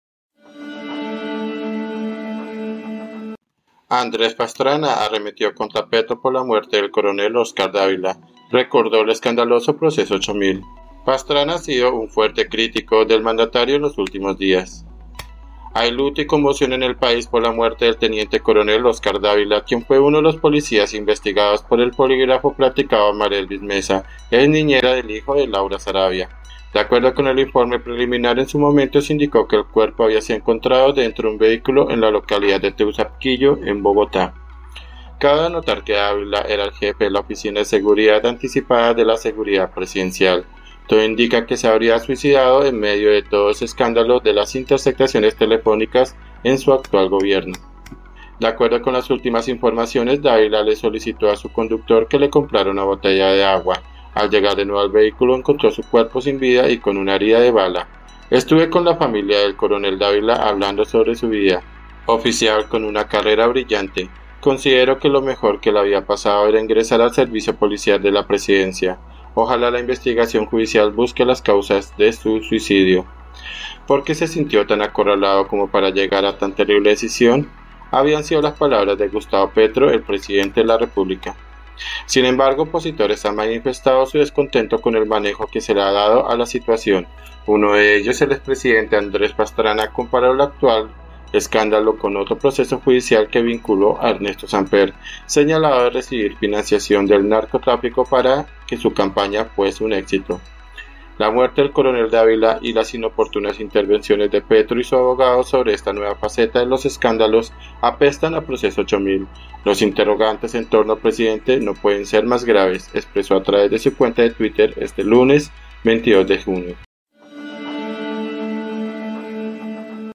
📌 ANDRÉS PASTRANA FUERTE RESPUESTA AL PRESIDENTE PETRO POR CASO CORONEL DÁVILA
Sin embargo, opositores han manifestado su descontento con el manejo que se le ha dado a la situación. Uno de ellos, el expresidente Andrés Pastrana, comparó el actual escándalo con otro proceso judicial que vinculó a Ernesto Samper, señalado de recibir financiación del narcotráfico para que su campaña fuese un éxito.